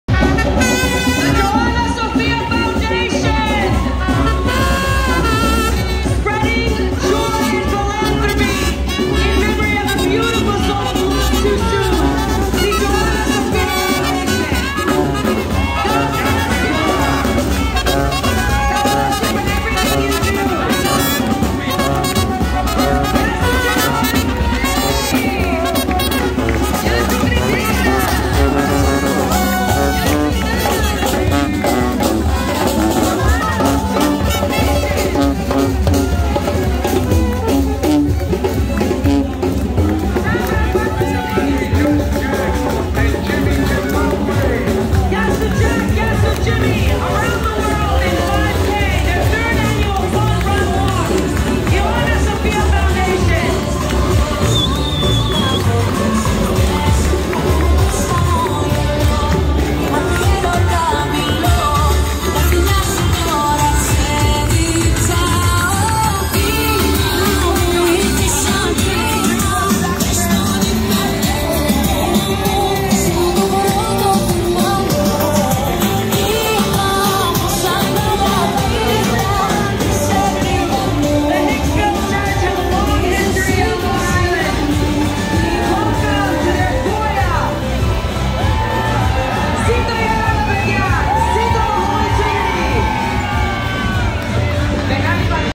Το ίδρυμα Joanna Sophia Foundation παρέλασε στην 5η Λεωφόρο της Νέας Υόρκης
Το Ίδρυμα Ιωάννα - Σοφία' ένα από τα νεώτερα φιλανθρωπικά ιδρύματα παρέλαση στην 5η Λεωφόρο και εξέπληξε το κοινό με τα χρώματα και γενικότερα την παράσταση.